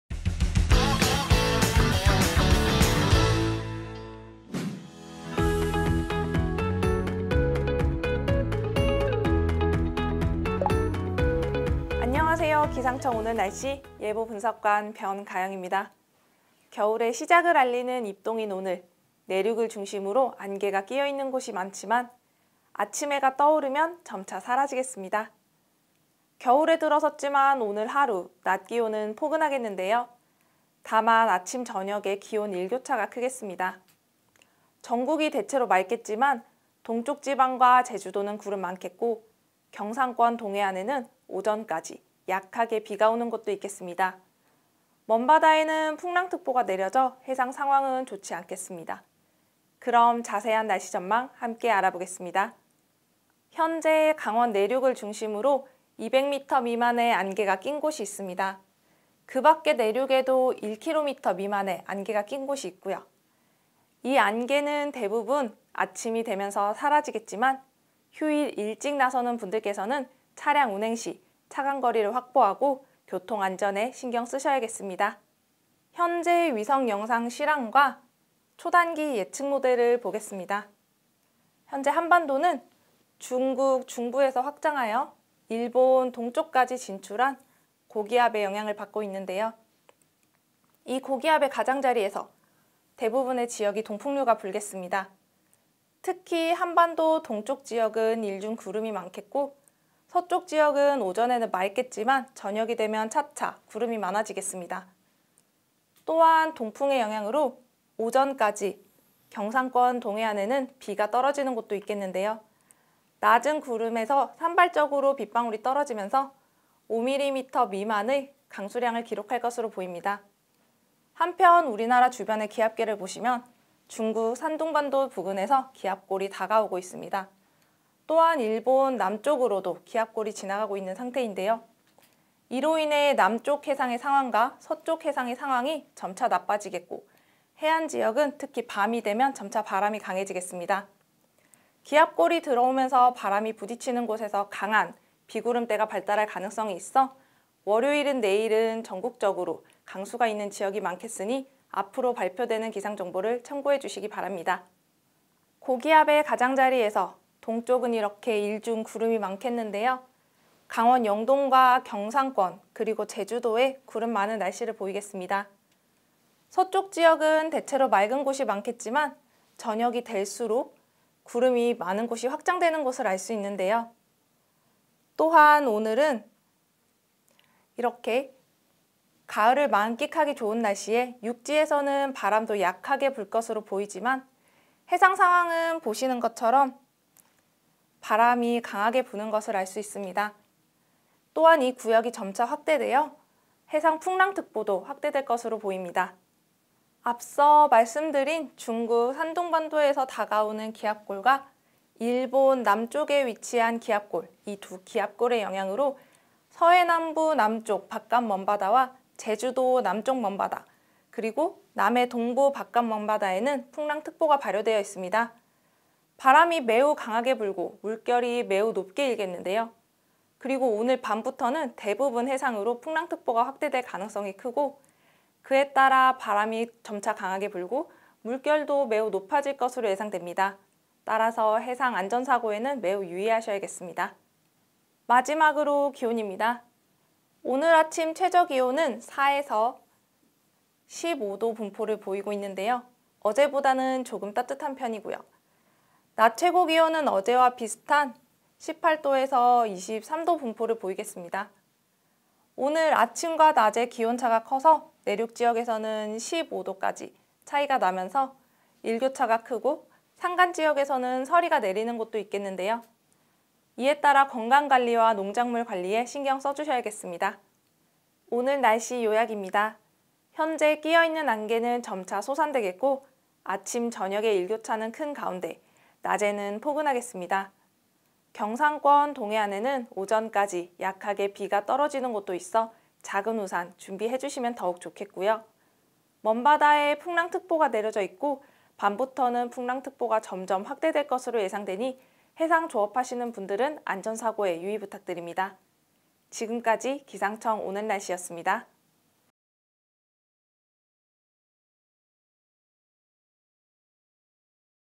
[오늘날씨] 전국 대체로 맑고 강원영동, 경상권, 제주도 구름많음, 경상동해안 빗방울 곳, 11월 7일 5시 기준
2021년 11월 7일 5시 예보를 해설한 오늘날씨입니다.

0:00 오프닝
0:05 오늘날씨 한눈에 보기
0:12 오늘의 날씨는?
0:53 시정분포도 / CCTV
1:17 위성영상 / 초단기 강수분포
2:35 하늘상태
2:57 풍속
3:18 특보현황
3:58 기온(최저 / 최고) / 일교차
4:28 오늘 날씨 요약

안녕하세요.
기상청 오늘 날씨 예보분석관 변가영입니다.
겨울의 시작을 알리는 입동인 오늘,
내륙을 중심으로 안개가 끼어있는 곳이 많지만,
아침 해가 떠오르면 점차 사라지겠습니다.
겨울에 들어섰지만, 오늘 하루
낮기온은 포근하겠는데요.
다만 아침저녁의 기온 일교차가 크겠습니다.
전국이 대체로 맑겠지만
동쪽지방과 제주도는 구름많겠고, 경상권동해안에는
오전까지 약하게 비가 오는 곳도 있겠습니다.
먼바다에 풍랑특보가 내려져 해상상황은 좋지 않겠습니다.
그럼, 자세한 날씨전망 함께 알아보겠습니다.

현재 강원 내륙을 중심으로
200m미만의 안개가 낀 곳이 있습니다.
그 밖의 내륙에도 1km미만의 안개가 낀 곳이 있고요.
이 안개는 대부분 아침이 되면서 사라지겠지만
휴일 일찍 나서는 분들께서는 차량 운행 시
차간거리를 확보하고 교통안전에 신경쓰셔야겠습니다.

현재 위성 영상 실황과 초단기 예측모델을 보겠습니다.
현재 한반도는 중국 중부에서 확장하여
일본 동쪽까지 진출한 고기압의 영향을 받고 있는데요.
이 고기압의 가장자리에서
대부분 지역이 동풍류가 불겠습니다.
특히 한반도 동쪽지역은 일중 구름이 많겠고,
서쪽지역은 오전에는 맑겠지만
저녁이 되면 차차 구름이 많아지겠습니다.
또한 동풍의 영향으로 오전까지
경상권동해안에는 빗방울이 떨어지는 곳도 있겠는데요.
낮은 구름에서 산발적으로 빗방울이 떨어지면서
5mm미만의 강수량을 기록할 것으로 보입니다.
한편 우리나라 주변의 기압계를 보시면
중국 산둥반도 부근에서 기압골이 다가오고 있습니다.
일본 남쪽으로도 기압골이 지나가고 있는 상태인데요.
이로인해 남쪽 해상의 상황과
서쪽해상의 상황이 점차 나빠지겠고,
해안지역으로 밤이 되면 점차 바람이 강해지겠습니다.
기압골이 들어오면서 바람이 부딪히는 곳에서
강한 비구름대가 발달할 가능성이 있어,
월요일인 내일은 전국적으로
강수가 있는 지역이 많겠으니
앞으로 발표되는 기상정보를 참고해 주시기 바랍니다.

고기압 가장자리에서
동쪽은 이렇게 일중 구름이 많겠는데요.
강원영동과 경상권 그리고 제주도에
구름많은 날씨를 보이겠습니다.
서쪽지역은 대체로 맑은 곳이 많겠지만,
이렇게 저녁이 될수록
구름이 많은 곳이 확장되는 것을 알수 있는데요.

또한 오늘은 가을을 만끽하기에 좋은 날씨에
육지에서는 바람도 약하게 불 것으로 보이지만,
해상 상황은 보시는 것처럼
바람이 강하게 부는 것을 알 수 있습니다.
또한 이 구역이 점차 확대되어
해상풍랑특보도 확대될 것으로 보입니다.

앞서 말씀드린 중국 산둥반도에서
다가오고 있는 기압골과
일본 남쪽에 위치한 기압골
이 둘 기압골의 영향으로
서해남부남쪽바깥먼바다와 제주도남쪽먼바다,
그리고 남해동부바깥먼바다에는
풍랑 특보가 발효되어 있습니다.
바람이 매우 강하게 불고 물결이 높게 일겠는데요.
오늘 밤부터는 대부분 해상으로
풍랑특보가 확대될 가능성이 크고,
그에 따라 바람이 점차 강하게 불고,
물결이 매우 높아질 것으로 예상됩니다.
따라서 해상 안전사고에는 매우 유의하셔야겠습니다.

마지막으로 기온입니다.
오늘 아침 최저기온은 4~15도 분포를 보이고 있는데요.
어제보다는 조금 따뜻한 편이고요.
낮 최고기온은 어제와 비슷한
18~23도 분포를 보이겠습니다.
오늘도 아침과 낮의 기온차가 커서
내륙지역에서는 15도까지
차이가 나면서 일교차가 크고
산간지역에서는 서리가 내리는 곳도 있겠는데요.
이에따라 건강관리와 농작물 관리에 신경써주셔야겠습니다.

오늘 날씨 요약입니다.
현재 끼어있는 안개는 점차 소산되겠고,
아침저녁의 일교차는 큰 가운데 낮에는 포근하겠습니다.
경상동해안에는 오전까지 약하게 비가 떨어지는 곳도 있어
작은 우산 준비해주시면 더욱 좋겠고요.
먼바다에 풍랑 특보가 내려져 있고,
밤부터는 점점 확대될 것으로 예상되니
해상 조업하시는 분들은 안전사고에 유의 부탁드립니다.
지금까지 기상청 오늘 날씨였습니다.